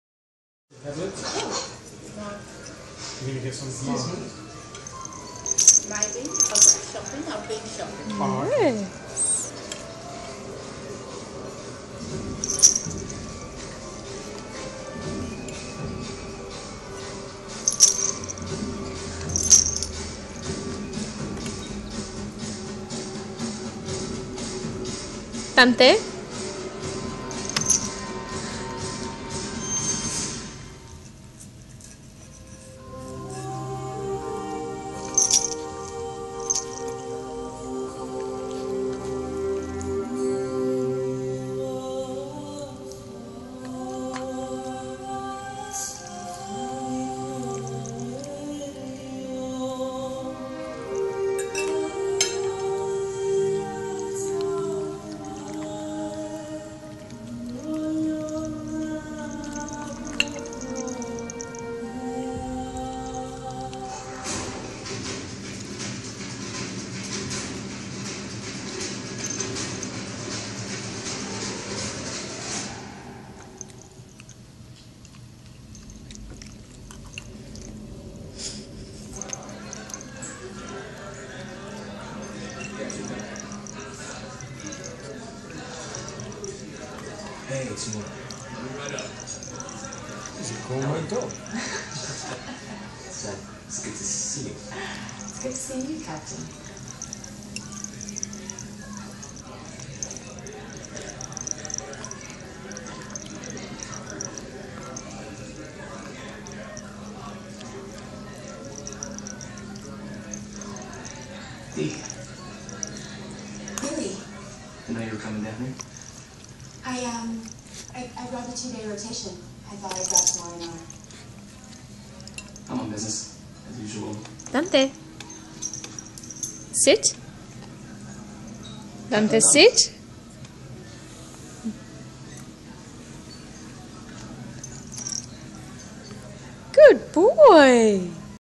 Dante the Fork Dog 2
Dante is back.. with more chicken and a special cameo from a lonely pea!